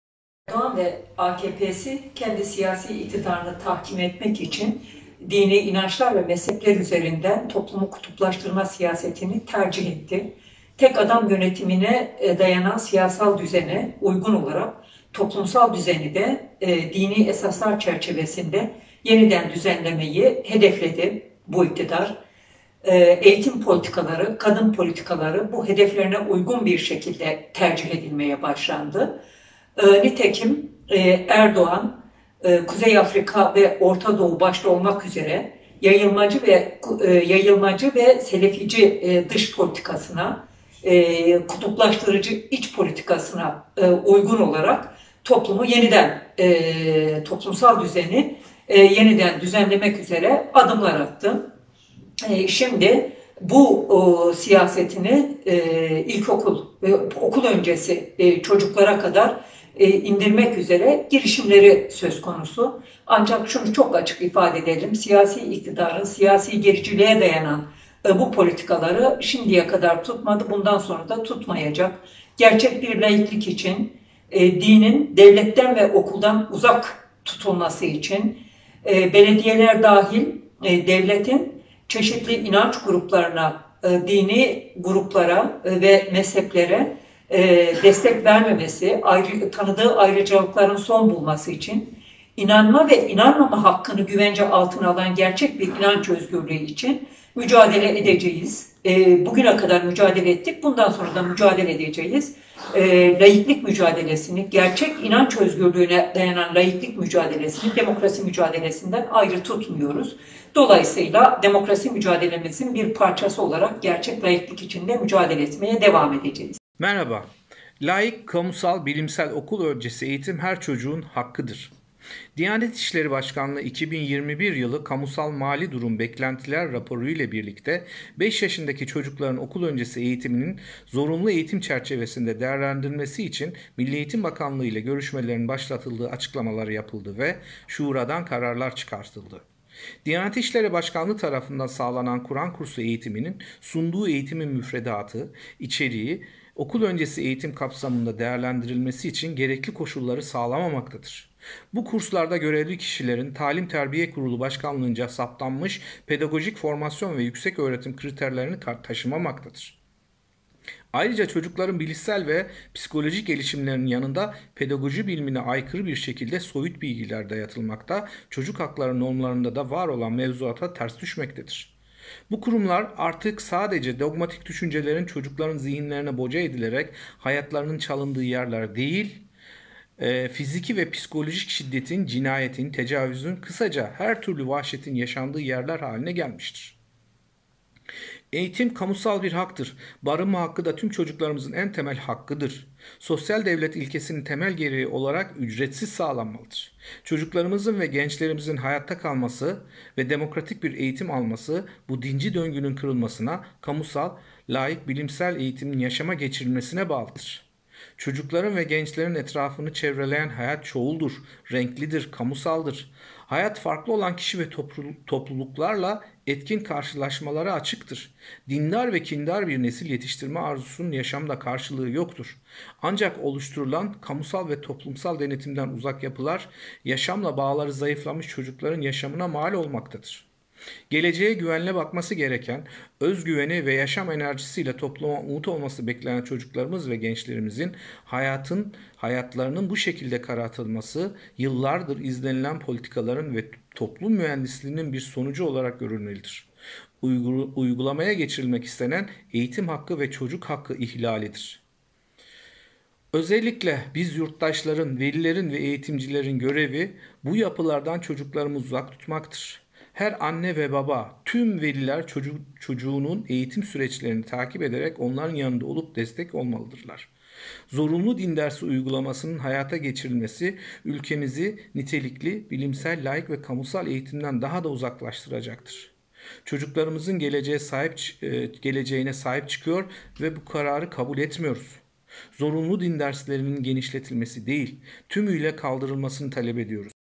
'Zorunlu din dersi, eğitim hakkı ve çocuk hakkı ihlalidir'
Ana sınıfına da zorunlu din dersi getirilmek istemesine karşı yürütülen imza kampanyası kapsamında konuşan Emek Partisi Genel Başkan Yardımcısı Selma Gürkan, Erdoğan’ın kendi iktidarını tahkim etmek için dini inançlar ve mezhepler üzerinden toplumu kutuplaştırma siyaseti izlediğini söyledi. Veli-Der Başkanı Ömer Yılmaz ise Diyanet İşleri Başkanlığı’nın MEB şurasında sunduğu tavsiye kararının yanlış olduğunu söyledi.